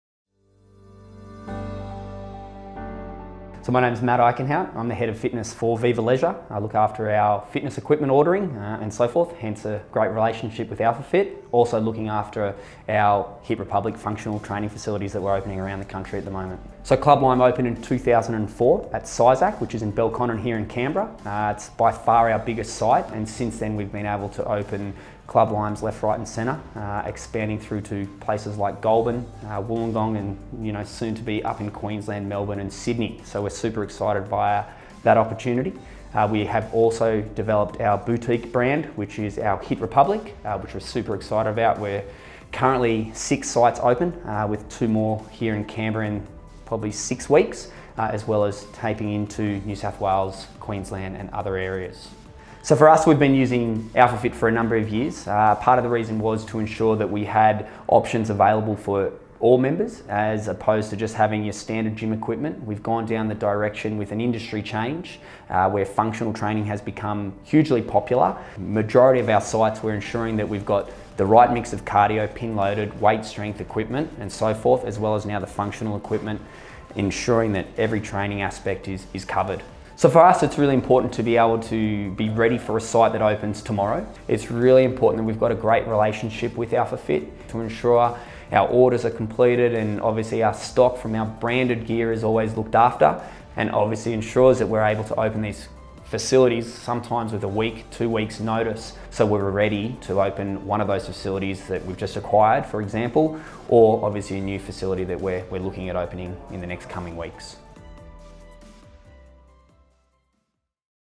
Viva Leisure: Why We Choose AlphaFit
Matt Eikenhout, Head of Fitness for Viva Leisure, explains why they choose AlphaFit as their premium equipment supplier.

Working together since the very first opening of Club Lime Belconnen in 2004, AlphaFit and Viva Leisure have established a strong relationship. Viva Leisure came to AlphaFit with a need for high quality, custom branded equipment for their commercial Club Lime facilities and also their boutique functional fitness spaces, Hiit Republic. The fitness industry is ever evolving and with that comes a greater demand for variety in the equipment provided to gym members. AlphaFit are trusted in the business to supply the right mix of gear to suit the needs of Viva Leisure’s club members and PTs.

With over 40 health clubs Australia wide currently in operation and more on the way, Viva Leisure and AlphaFit know the future is bright for their growing partnership.